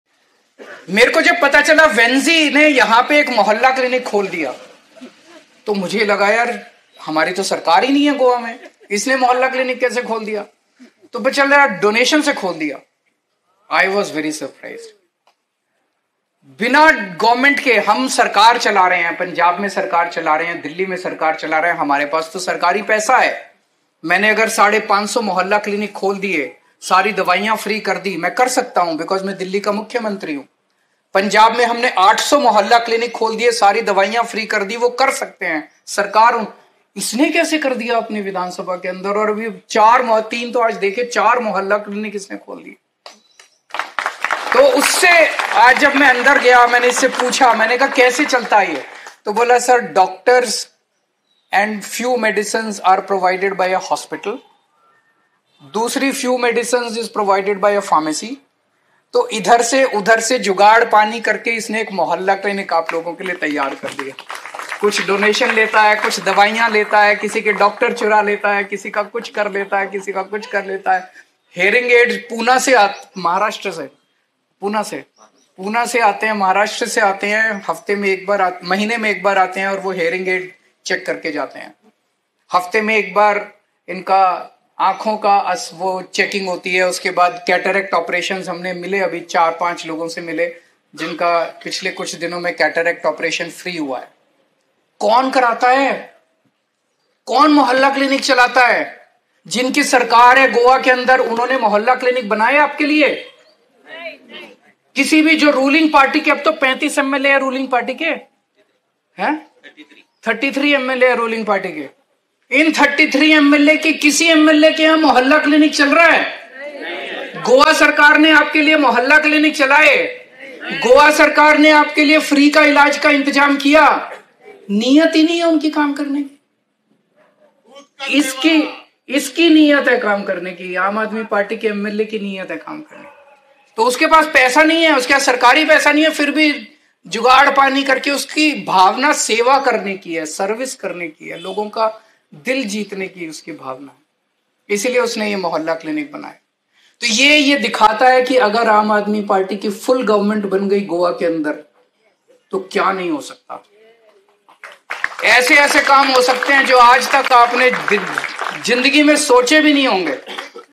AAP's MLA Venzy Viegas Runs 4 Mohalla Clinics in Goa with Donations | News18 | English News
Goa's AAP MLA Venzy Viegas is spearheading the operation of four mohalla clinics in his constituency, funded entirely by donations. Amidst this, the AAP government in Goa emphasizes people's health. Chief Minister Kejriwal highlights the contrast, questioning BJP's contribution with 33 MLAs in Goa and commends AAP's commitment to public service through initiatives like MLA Venzy Viegas's selfless efforts.

n18oc_india